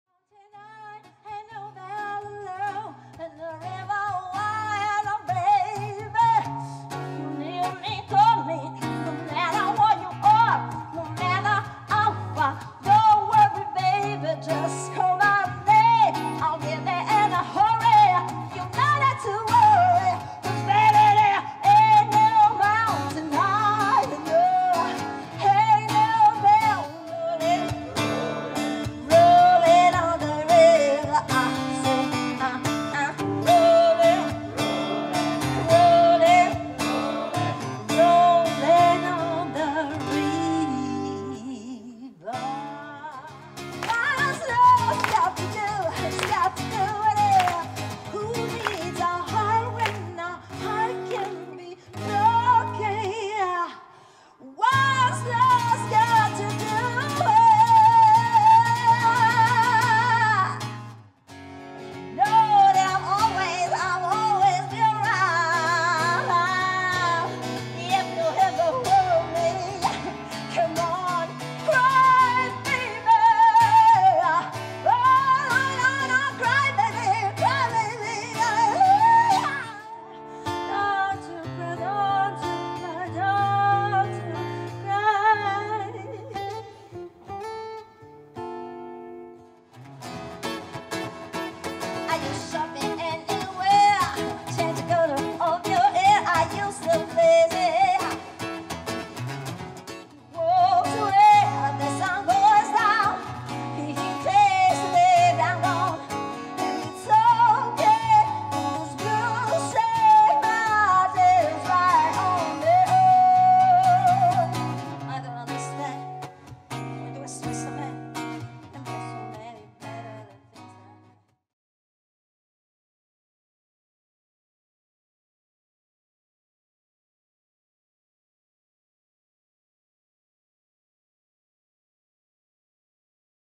Woman in Rock Ketty Roselli videoclip
Clip video dello spettacolo Woman in Rock di Ketty Roselli fatto a Roma il 4 luglio 2022. Riprese a mano libera effettuate con una Canon EOS R5 + RF 100-400 F5.6-8